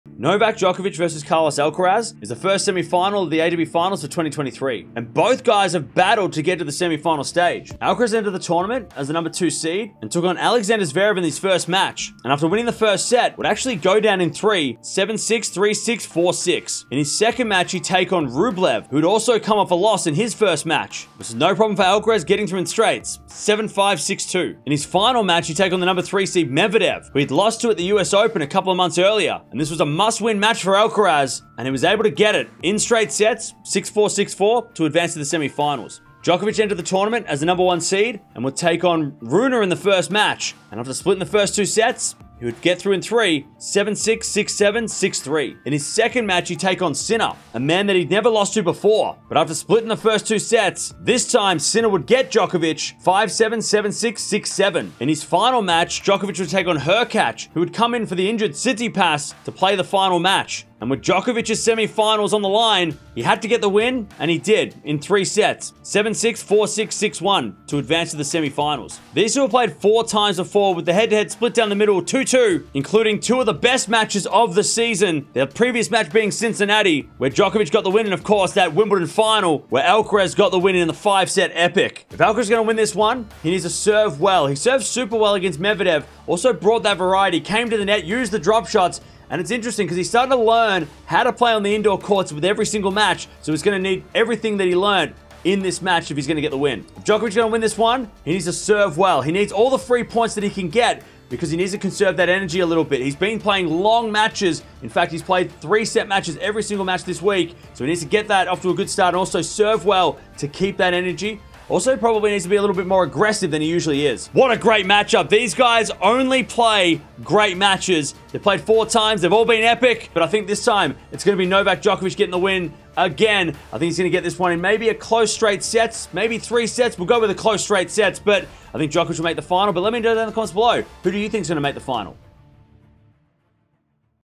Djokovic vs Alcaraz | ATP Finals 2023 Semi Final | Tennis Prediction
Novak Djokovic will take on Carlos Alcaraz in the Semi Final at the Nitto ATP Finals for 2023 in Turin. Djokovic had a nervous wait as Sinner and Rune decided his fate while Alcaraz destroyed Medvedev to take his place. Who will make the final?

0:00 | Intro
0:07 | Carlos Alcaraz
0:36 | Novak Djokovic
1:09 | Head to Head
1:22 | Keys to the Match
1:54 | Prediction